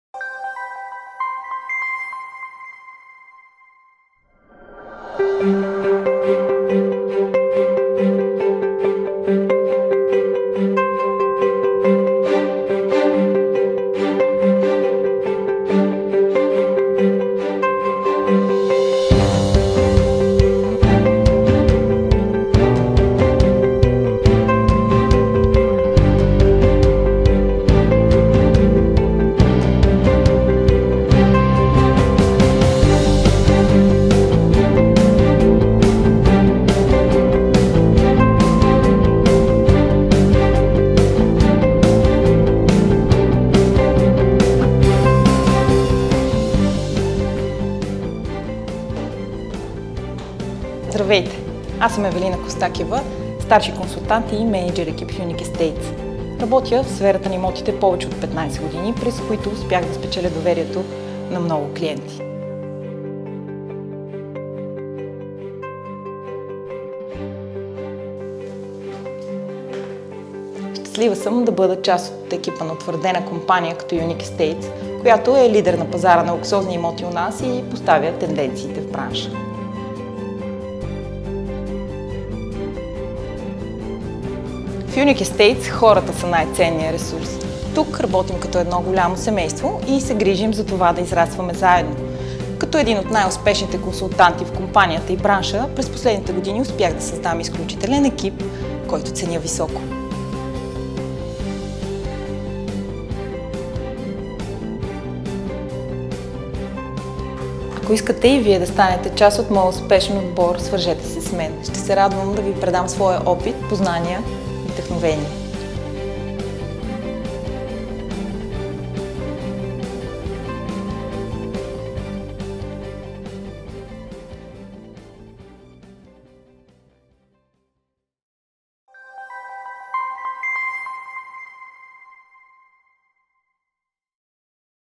Станете част от екипа на Unique Estates!
15 години отговаряме на международни стандарти. 15 години свързваме хората с мечтания им дом. 15 години сме лидери на пазара на луксозни имоти. Ако искаш да знаеш къде ще бъдем след още 15 години, присъедини се към нас. Нека извървим този път заедно!